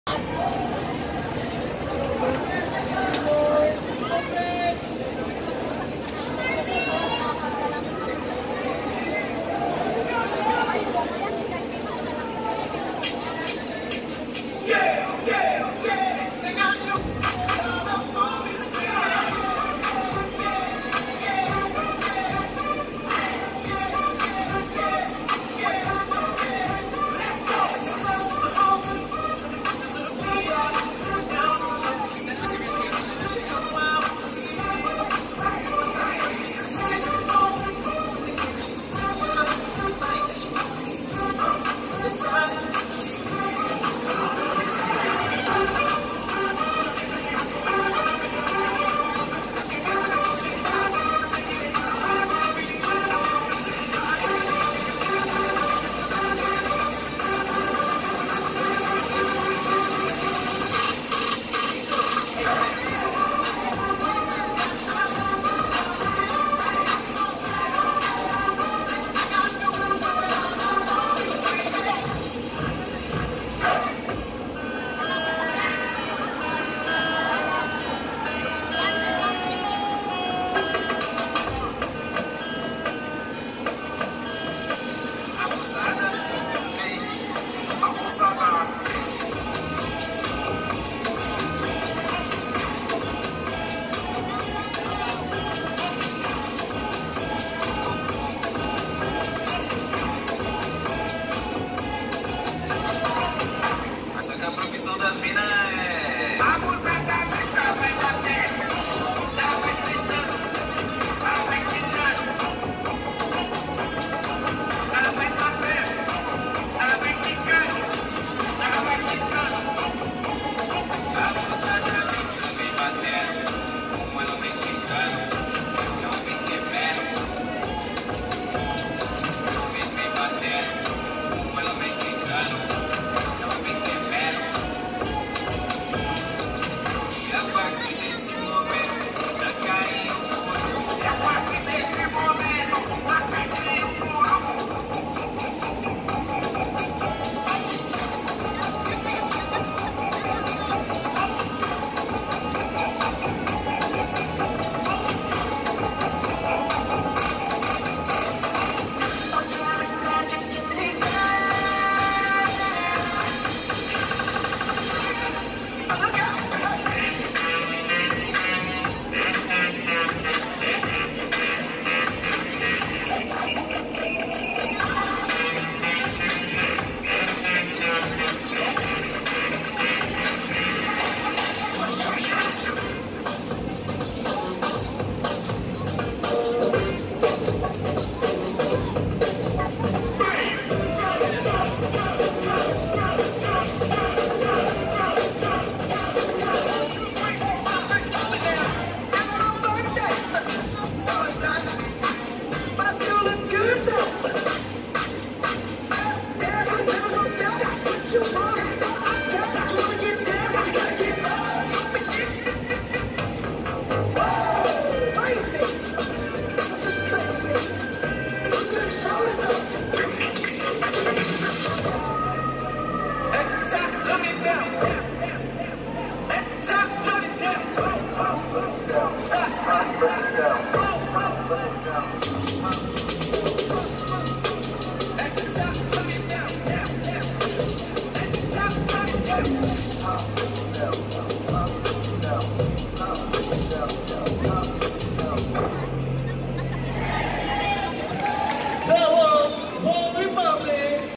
BATTLEGROUNDS PHILIPPINES 2019 - ONE REPUBLIC
BATTLEGROUNDS MINDANAO LEG
WORLD SUPREMACY BATTLEGROUNDS

**YOUNG DIV**
1.] BREAKIN POINT - CHAMP
2.] CROSSOVER GIRLS - 2ND
3.] D'NEWBIES CREW - 3RD

**VARSITY DIV**
1.] NEW CHAPTER - CHAMP
2.] EMINENT - 2ND
3.] AG STALLION - 3RD
4.] OFFLIMITZ - 4TH
5.] THE ROOKIES - 5TH

**OPEN DIV**
1.] OVERLOAD CREW - CHAMP
2.] BODY EXTREME - 2ND
3.] EYES ON ME - 3RD
4.] THE YOUNG GUYZ ACADEMY - 4TH
5.] CITY BROTHERS - 5TH

**MONSTER DIV**
1.] TEAM INDAYOG - CHAMP
2.] E.M.N.T. - 2ND
3.] CROSSOVER FAMILY - 3RD
4.] SOVEREIGN - 4TH
5.] ELITES - 5TH